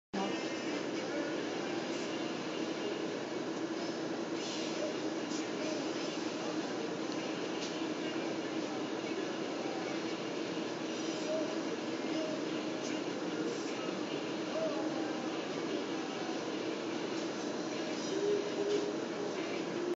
basAR: Using a Toten
How to create toten with augmented reality authoring tool.